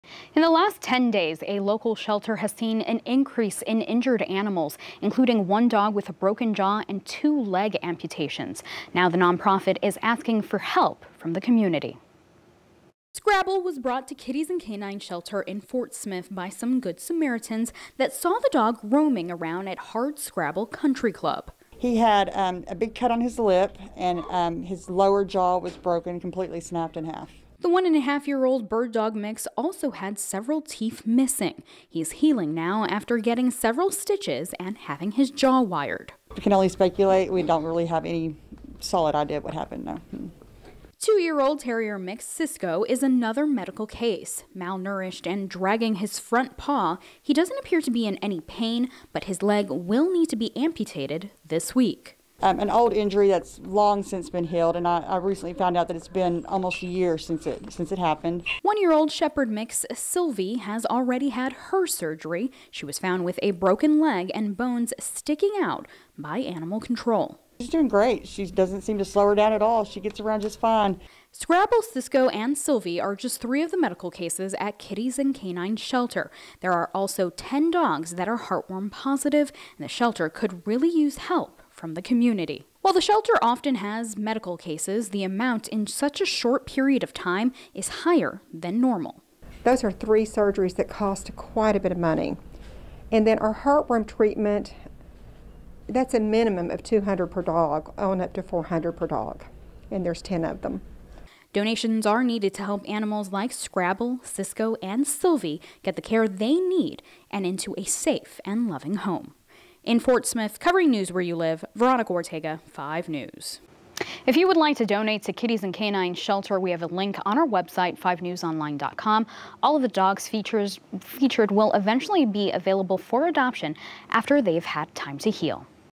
Kitties and Kanines Shelter Needs Help After a Spike in Medical Cases
In the last ten days, the shelter has seen an increase in injured animals including one dog with a broken jaw and two leg amputations.